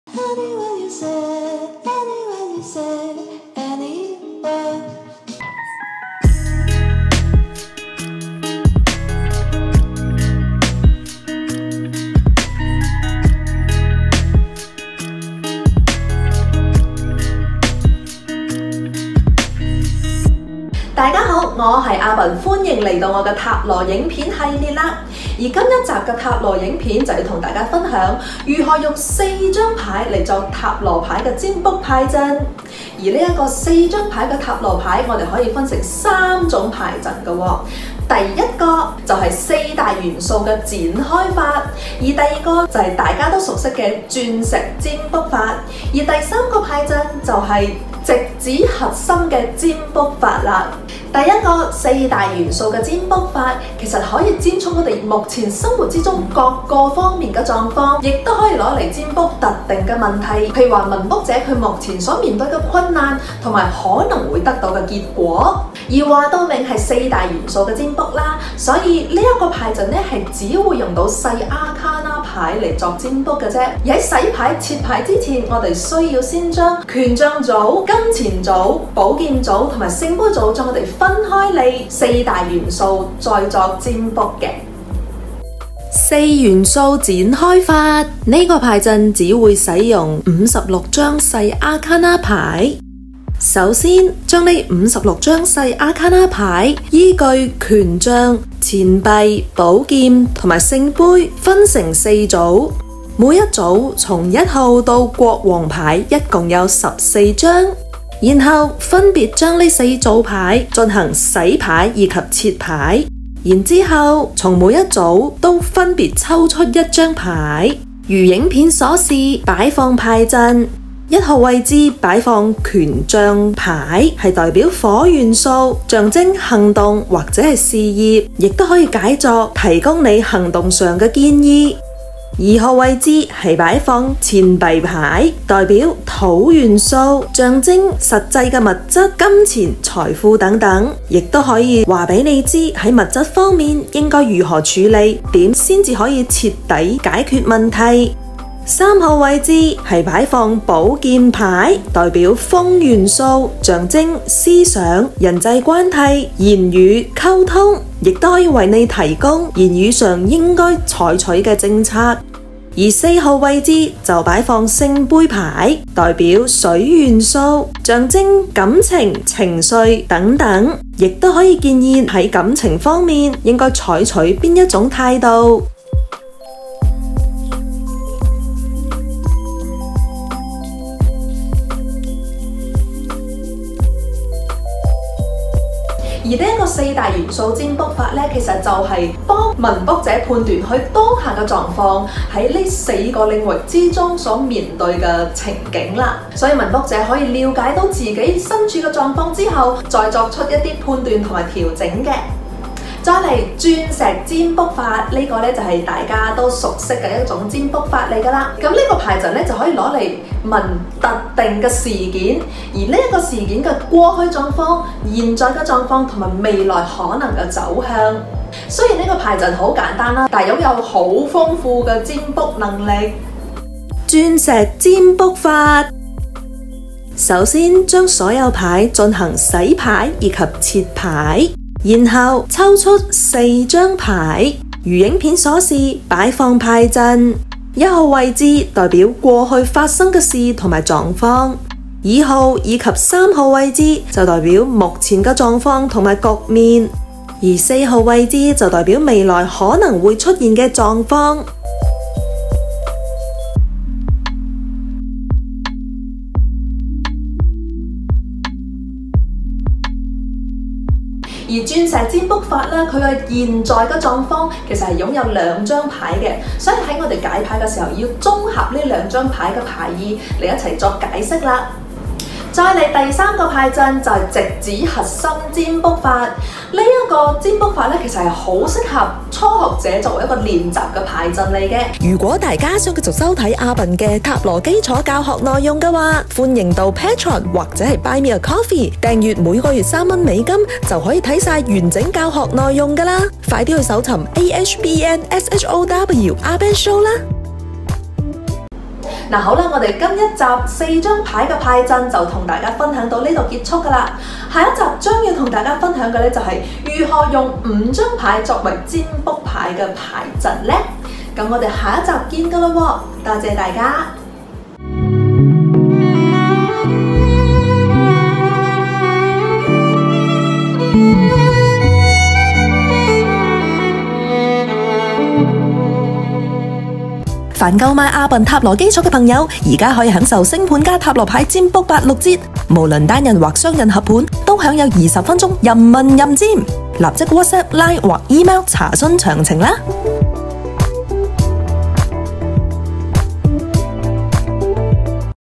4張牌「占卜牌陣」🃏【塔羅基礎】- 41
這一集，分享的是利用「4張牌作占卜」，【完整版影片】共分享三個牌陣。

00:22 牌陣介紹
00:52 牌陣一
03:23 牌陣二
04:32 牌陣三

✅ 或聯絡：
Bernice

～推薦觀看～

～～更多【影片系列】～～